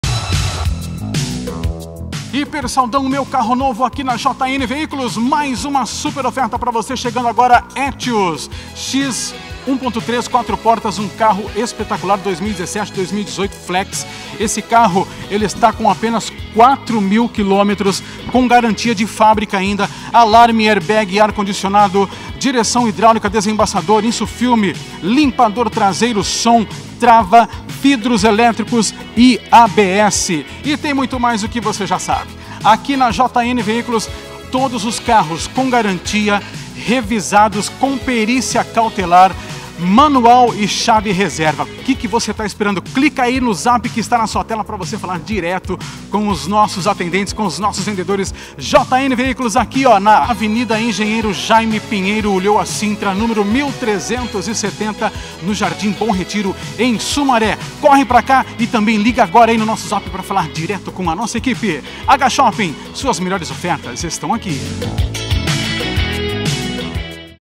JN VEÍCULOS - TOYOTA ETIOS 1.3 2017
Agora você pode ter o seu Etios na sua garagem. Na semana de Megas promoções da JN Veículos você pode parcelar o seu carro em até 48 vezes. Ligue agora mesmo para nossa loja em Sumaré e saiba mais: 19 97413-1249